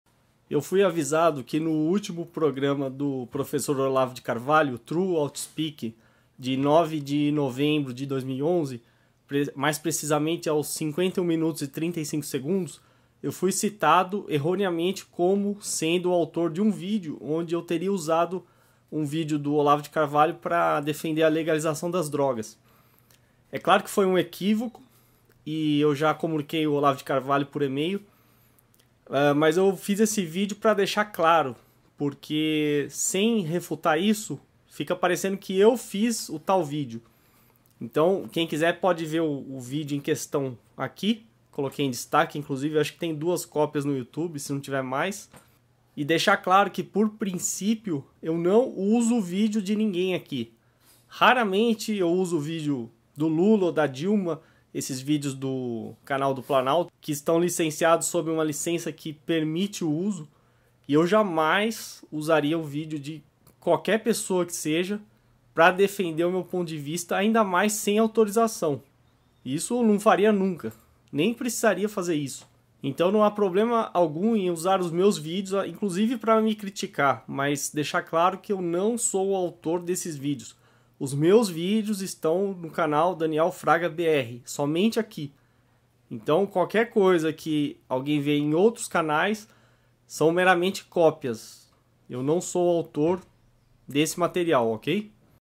True Outspeak 9/11/11: não sou o autor do vídeo citado
True Outspeak - 9 de novembro de 2011 (51:35)

Resposta pública ao Olavo de Carvalho para esclarecer que eu NÃO sou o autor do vídeo citado no programa e jamais usaria os vídeos do Olavo de Carvalho para essa finalidade ou sem prévia autorização.

Resposta de Olavo de Carvalho:
True Outspeak - 30 de novembro de 2011 (parte 2) - 16:00